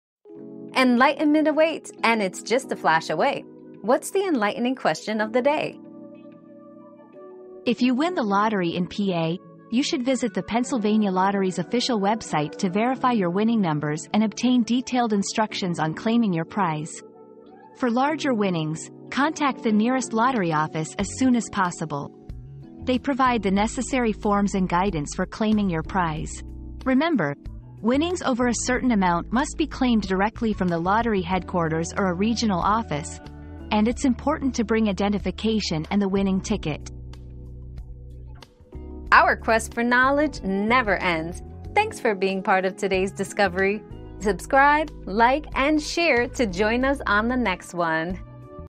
Where do I go if I win the lottery in PA?
Claiming Lottery Winnings in PA: What You Need to Know! 👉 Lottery Winnings in PA 👉 Discover the steps to claim your lottery winnings in Pennsylvania, from verifying your numbers online to contacting the nearest lottery office for larger prizes. Don't forget your identification and winning ticket!

Laura S. Harris (2024, April 15.) Where do I go if I win the lottery in PA?
    🌐 AskAbout.video